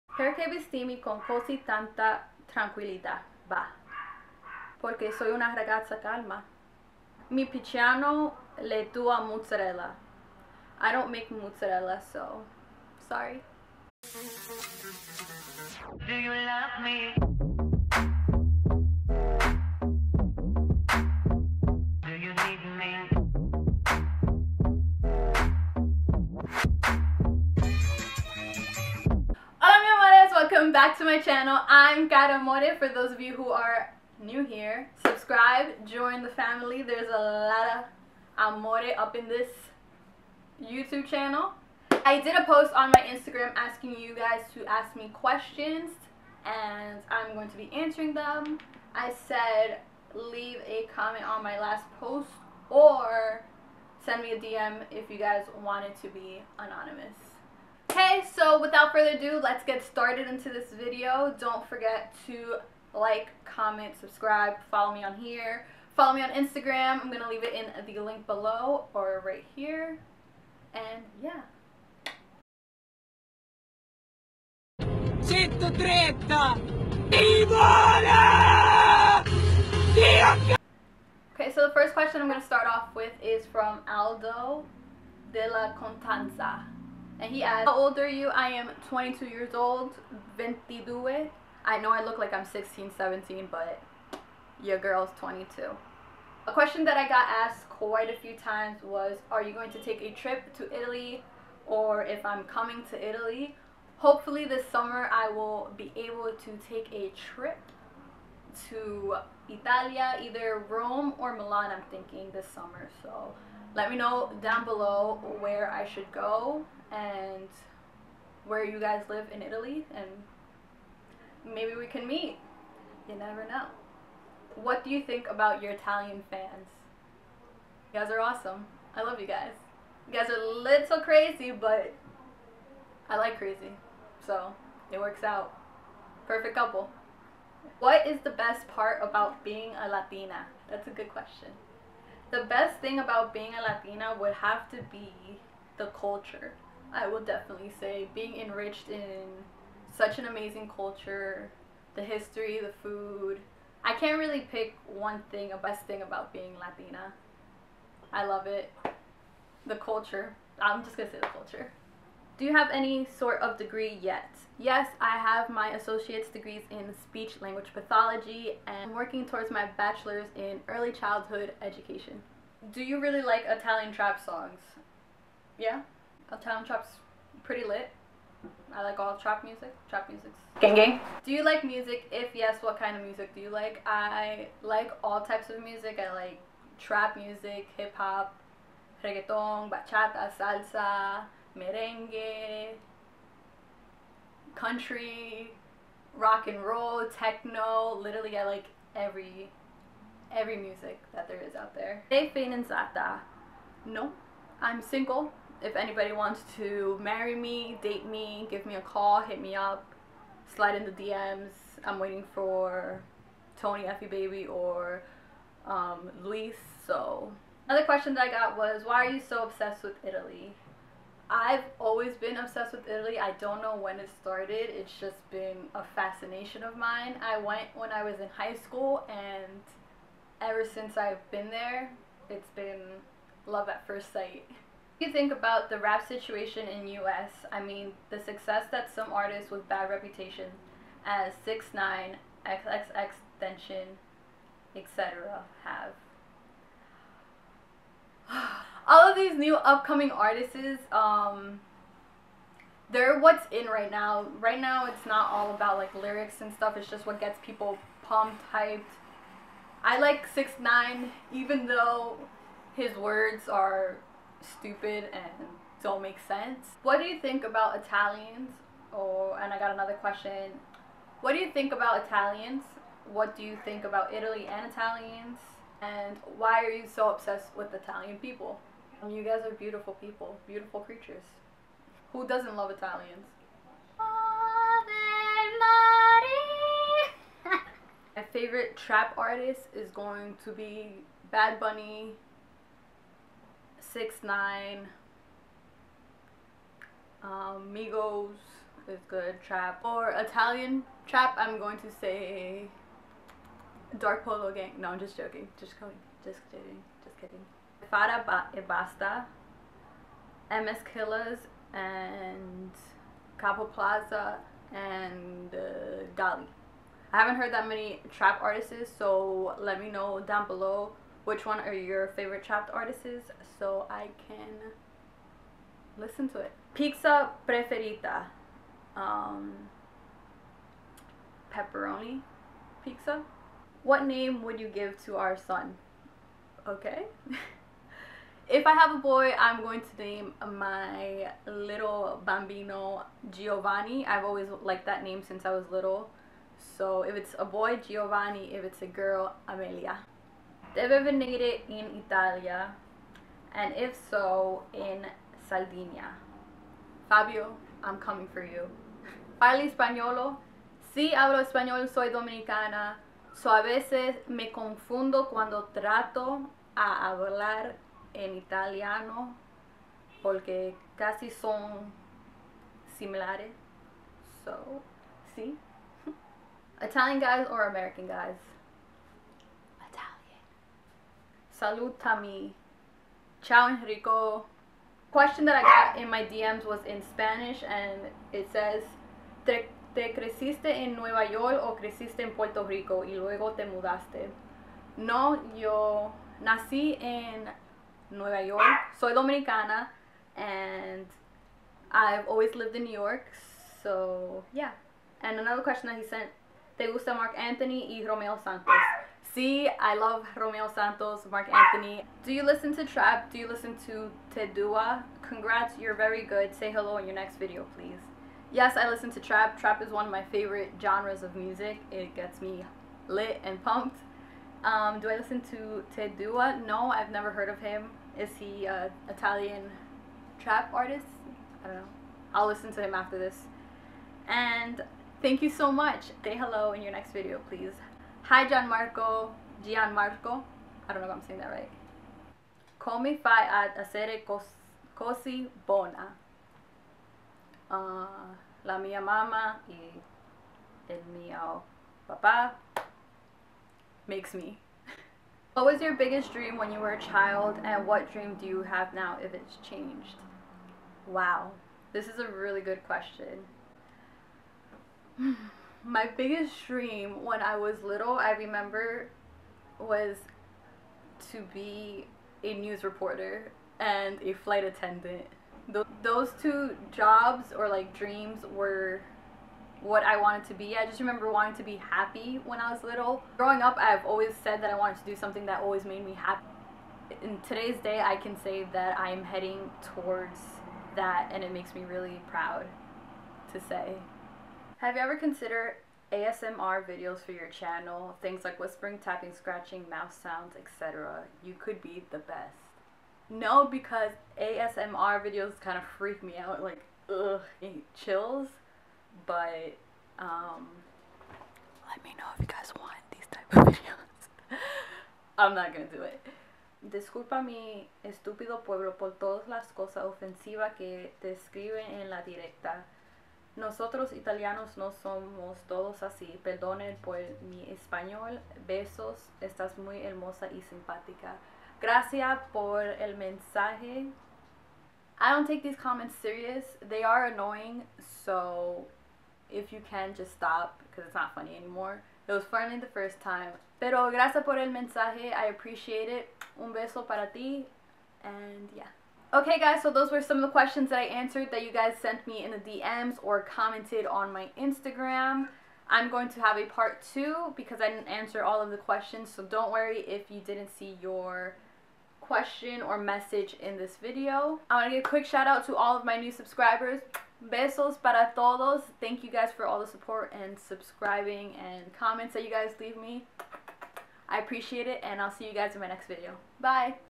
MY FIRST Q&A ( KAYRA_AMORE) MY AGE, MOVING TO ITALY, FAVORITE TRAP ARTIST & MORE !!!
💕Hola mi amores💕

Camera I used to film this video :Nikon D5100